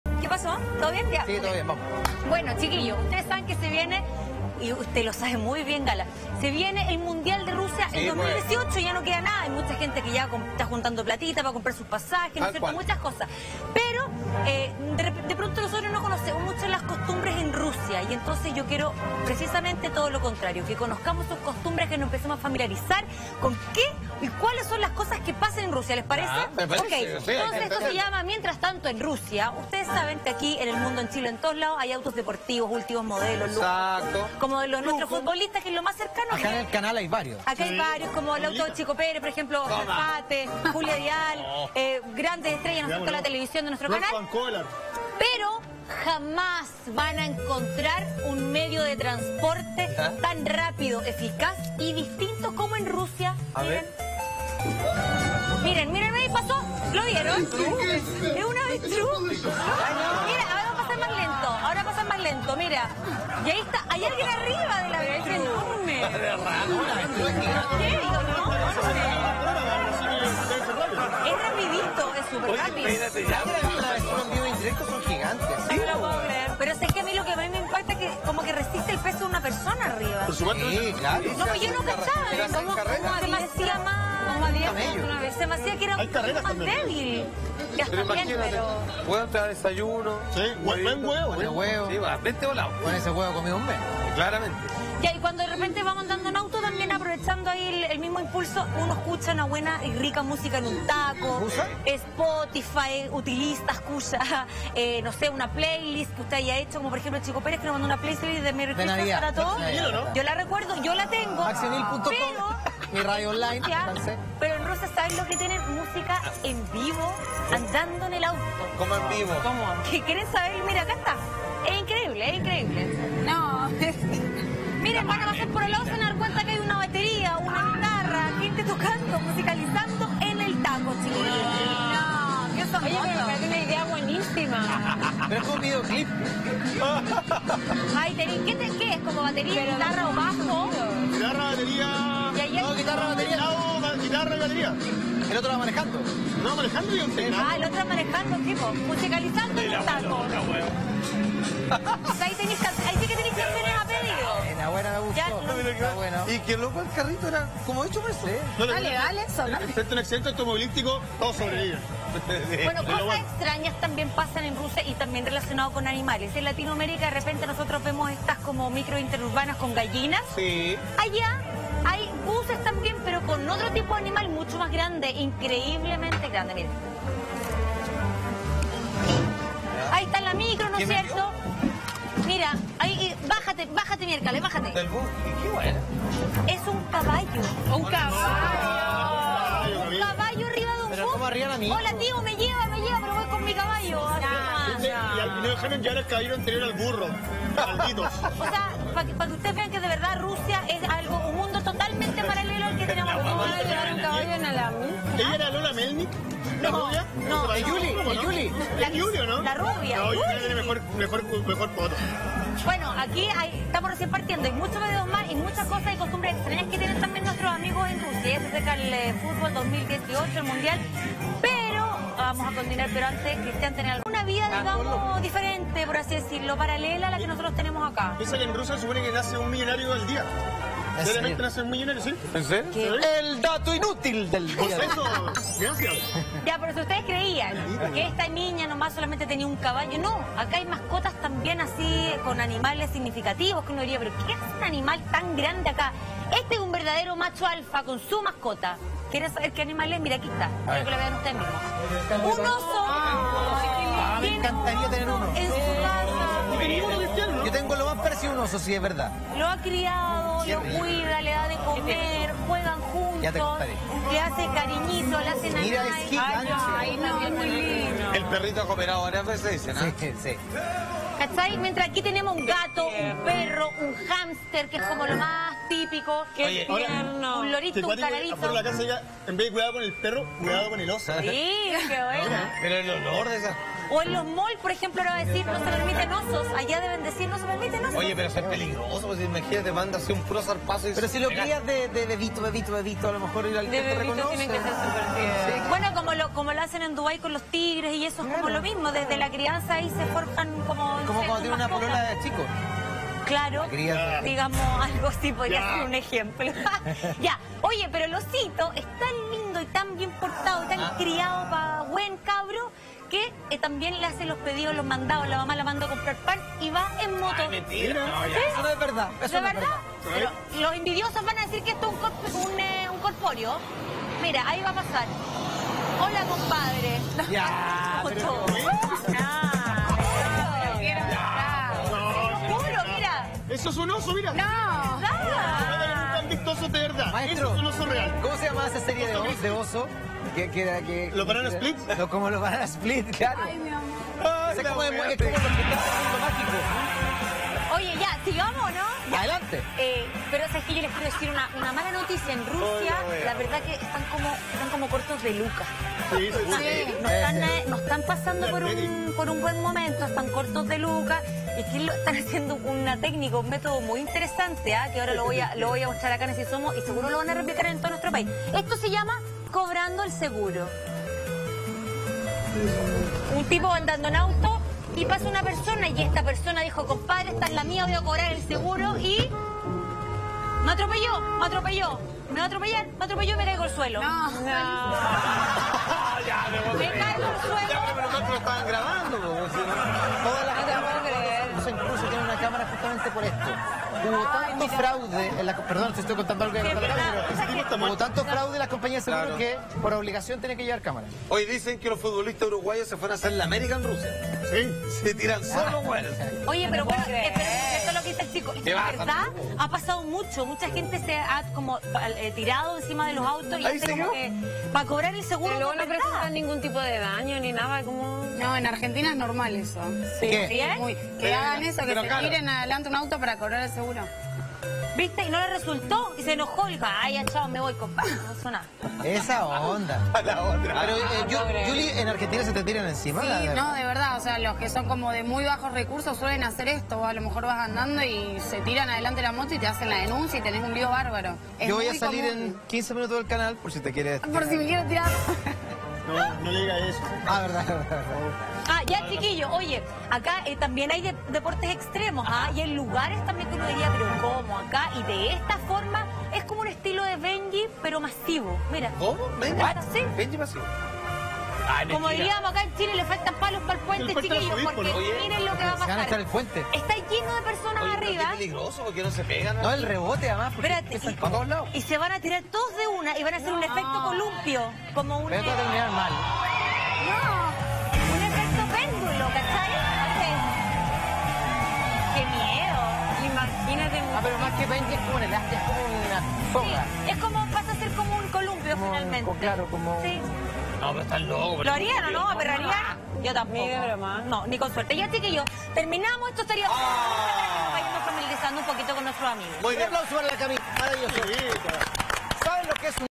Los locos videos de los rusos que te dejarán con la boca abierta
Camila Andrade nos trae estos divertidos virales, en donde los rusos son los protagonistas.